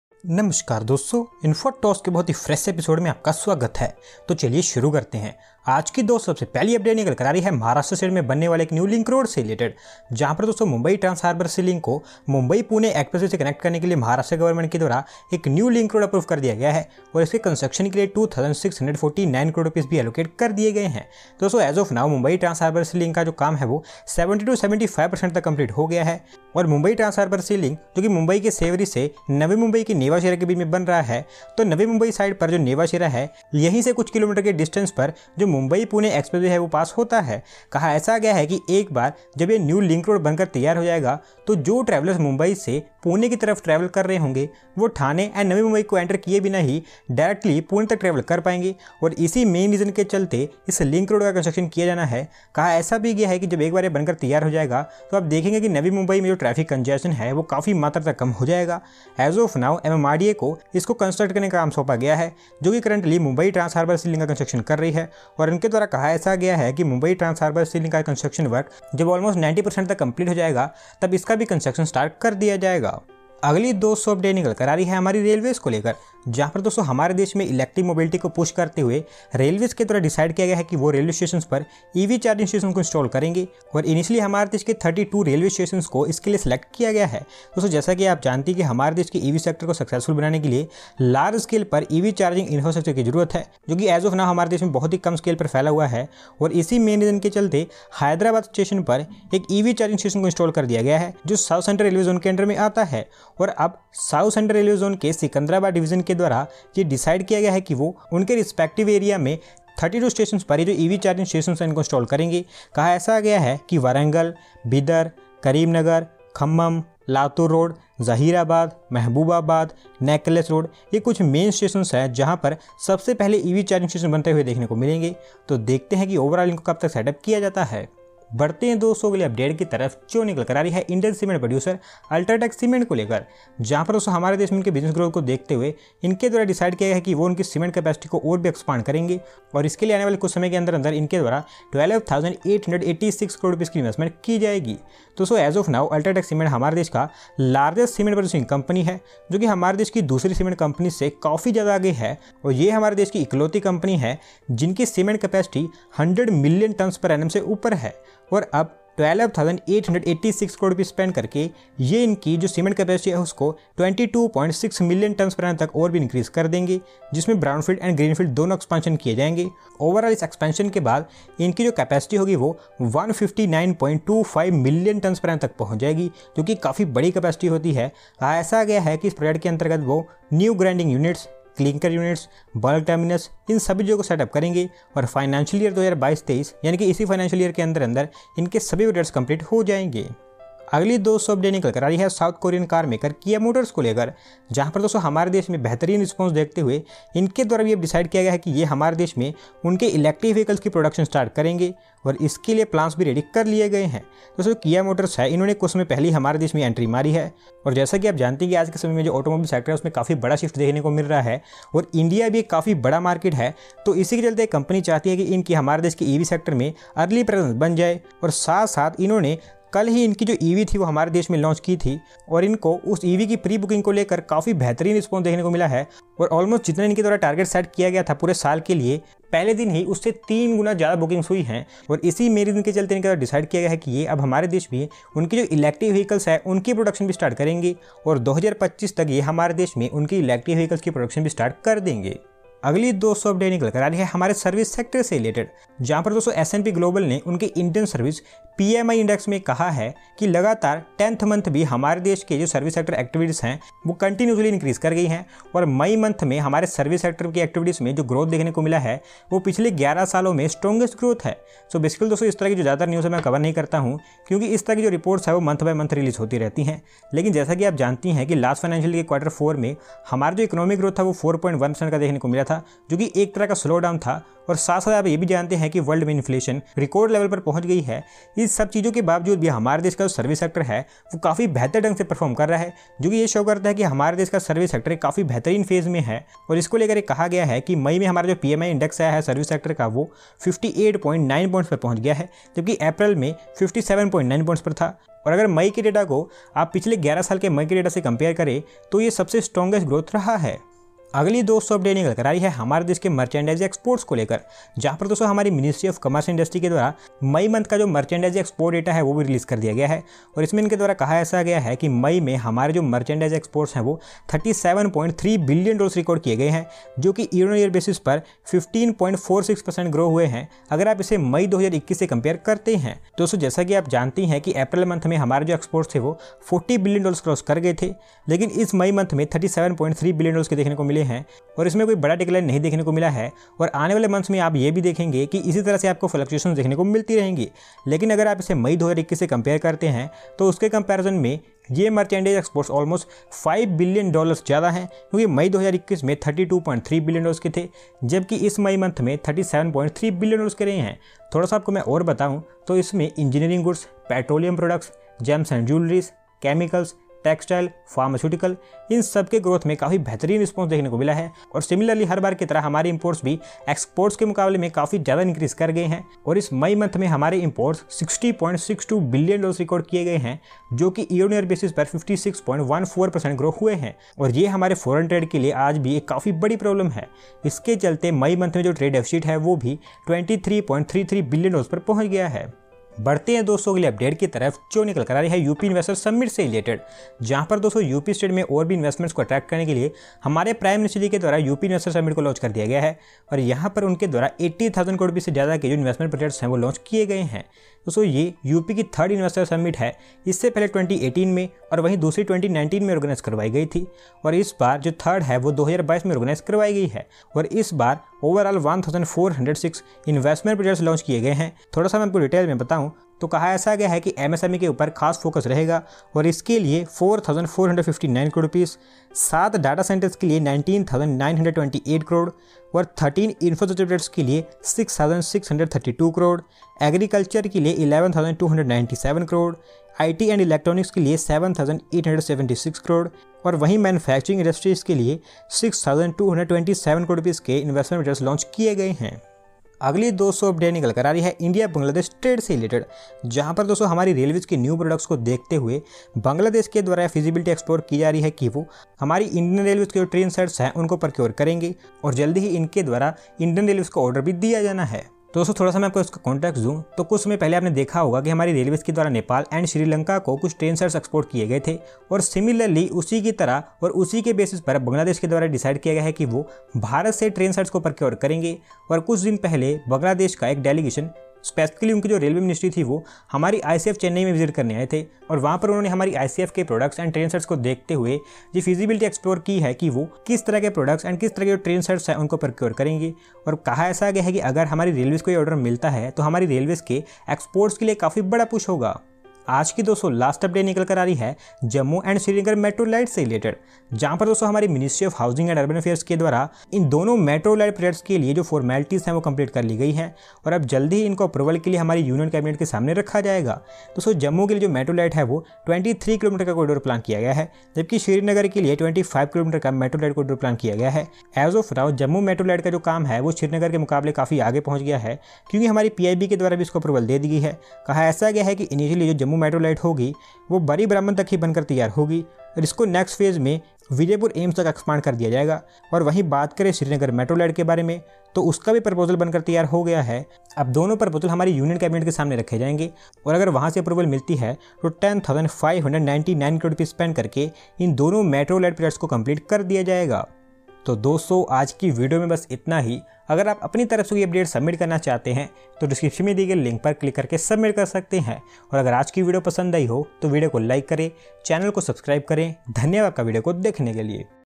Infratalks #499 - Service Sector Breaks 11 yr Record, J&K Metrolite Soon, KIA EV Plant, UP Investor
Timestamps
00:00 - Introduction
00:05 - New Link Road Mumbai
01:13 - 32 EV Charging Stations
02:08 - Ultratech Expansion
03:10 - KIA EV Plant India
04:02 - Services Sector 11 Yrs Strongest
05:00 - Exports Goes Up Again(Advaitya)
06:22 - UP Investors Summit
07:19 - Rail Exports to Bangladesh
08:09 - J&K Metro Lite